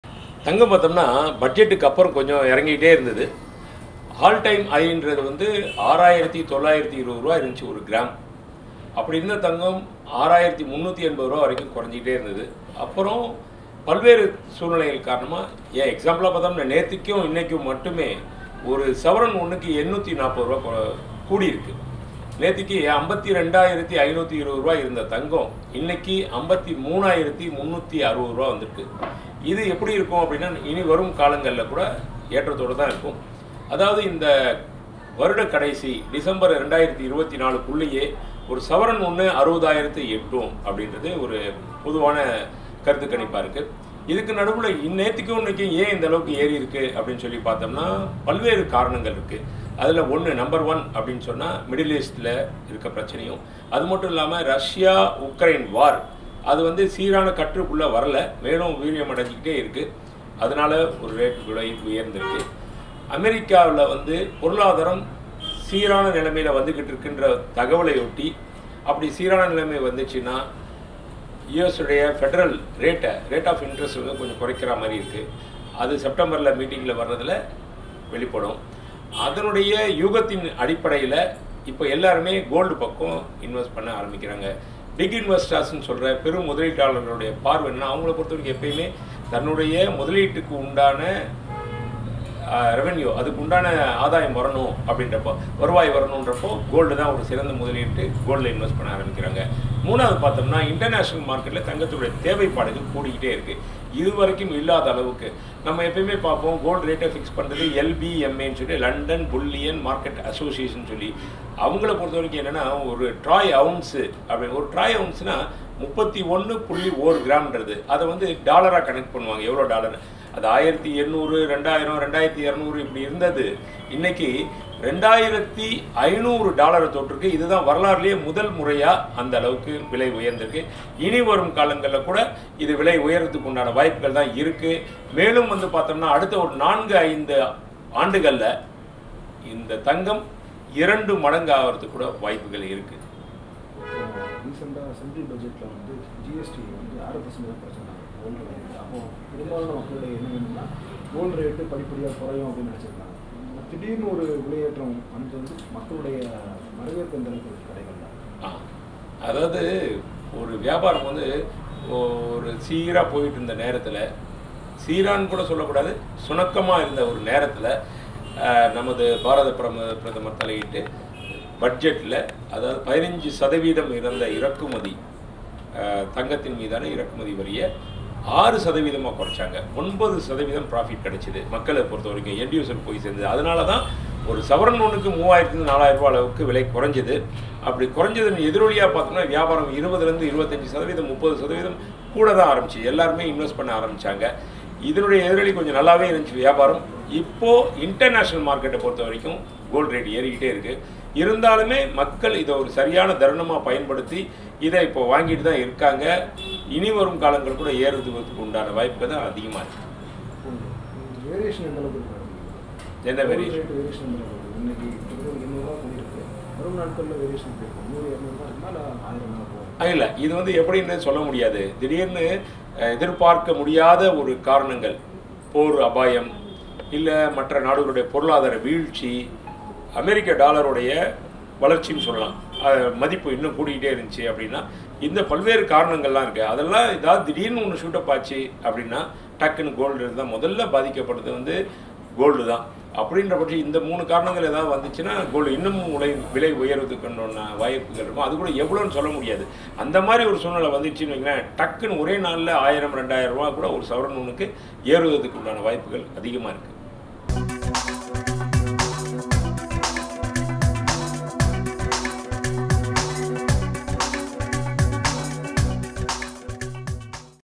மீண்டும் அதிரடி காட்ட தொடங்கிய தங்கம் விலை..இந்தாண்டு இறுதிக்குள் 60,000 தொடுமாம்..?
பட்ஜெட்டுக்கு பிறகு இறங்குமுகமாக இருந்த தங்கம் விலை மீண்டும் அதிரடி காட்ட தொடங்கி விட்டது..? இந்தாண்டு இறுதிக்குள் 60,000 தொடுமாம்..? தங்க நகை வியாபாரிகள் சங்க நிர்வாகி பேட்டி